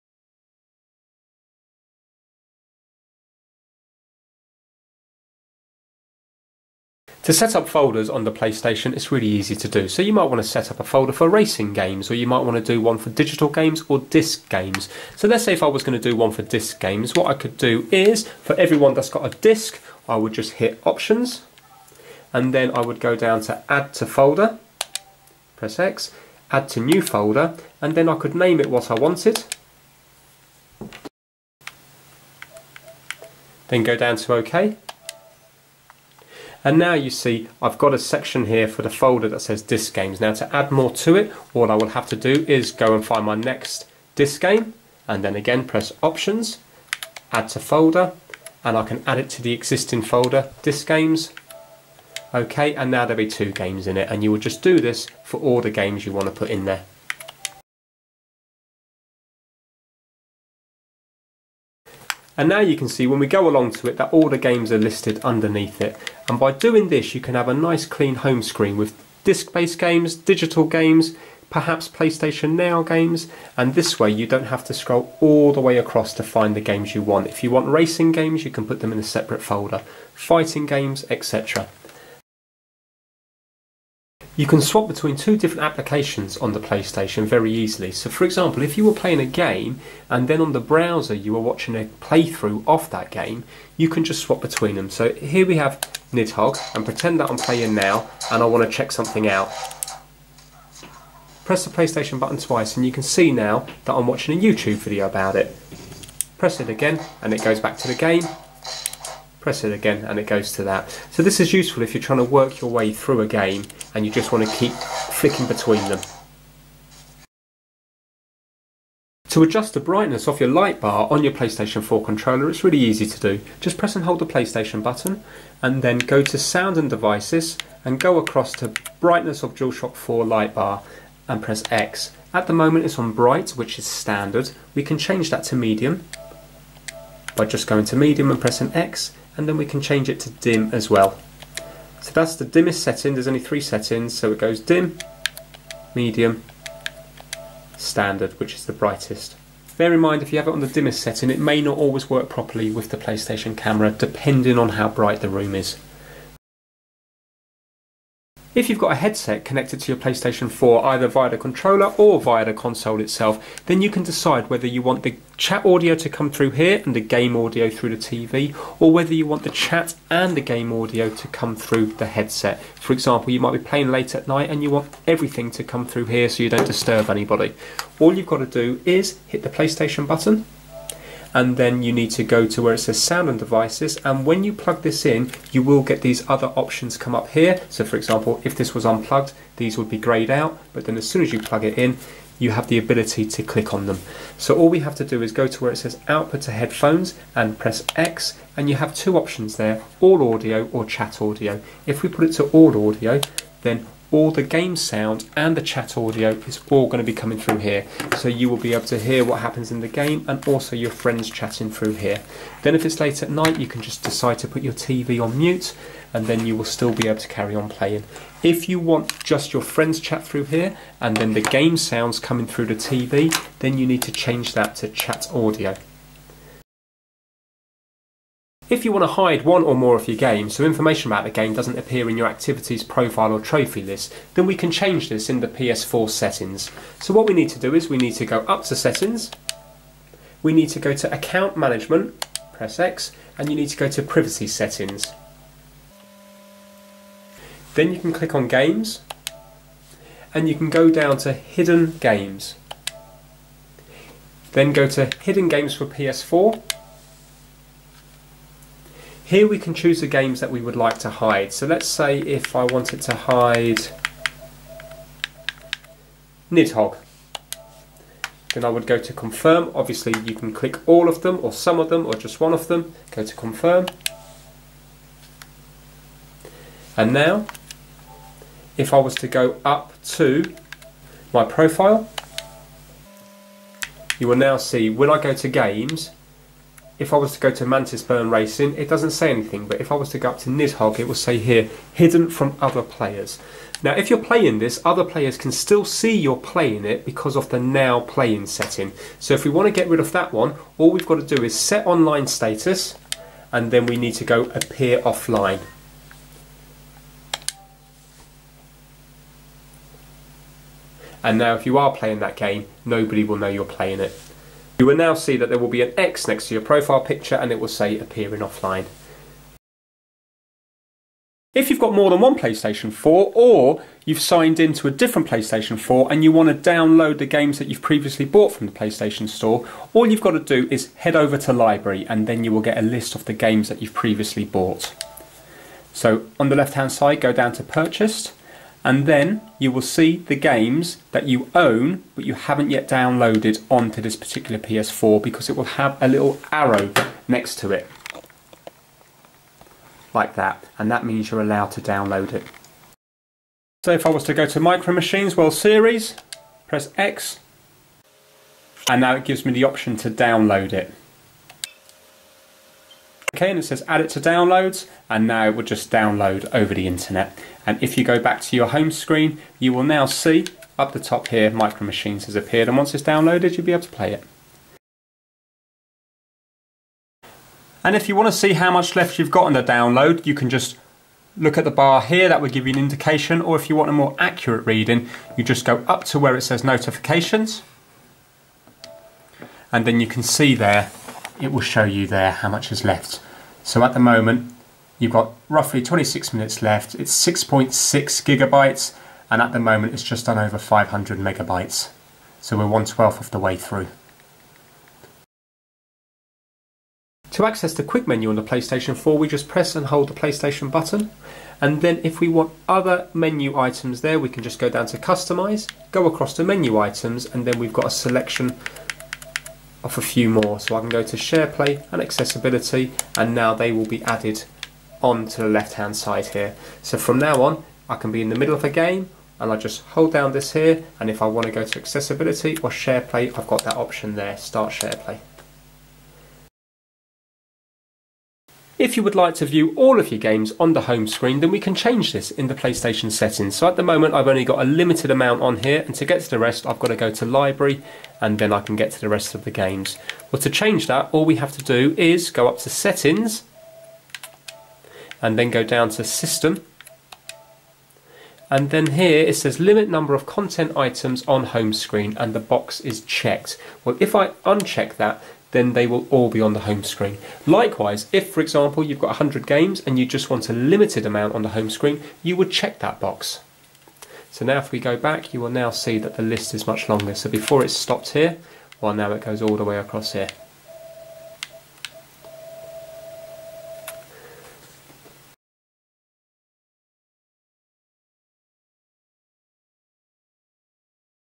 PART 2: 9 Useful PS4 Settings
Hi, this video shows you 9 useful settings on the PlayStation 4 which may help you out. I have also done a PART 1 video with another 9 settings which you may like.
The video shows things like adjusting the brightness on the controller light bar, how to hide games and creating folders etc.